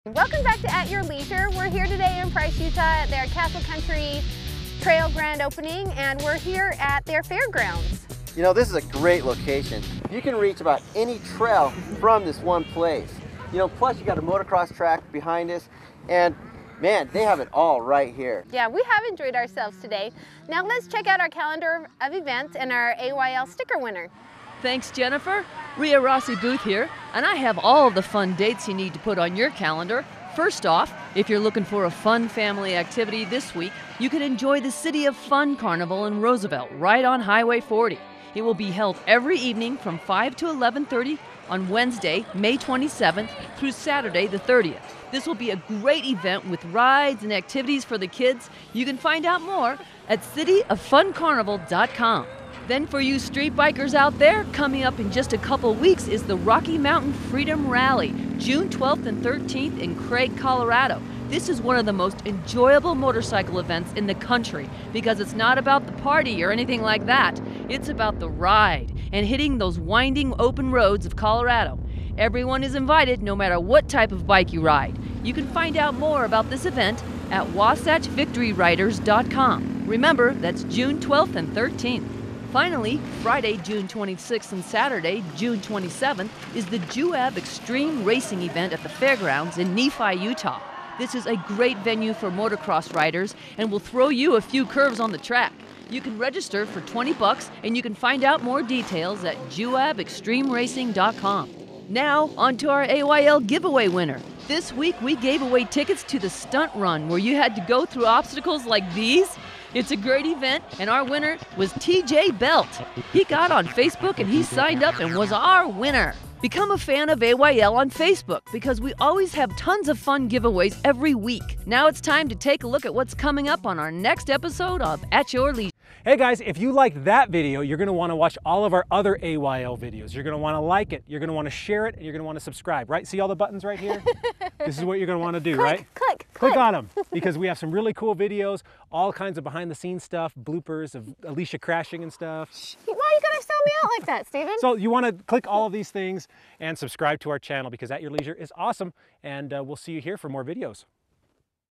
City of Fun - Rocky Mountain Freedom Ralley - Juab Xtreme Racing
At Your Leisure is your source for all the cool things to do in the great outdoors. In this week's travel planner Ria covers the City of Fun Carnival, the Rocky Mountain Freedom Rally, and the Juab Xtreme Racing Event, and announce this weeks sticker winner.

2469 E. 7000 S. Suite 110
Salt Lake City, Utah 84121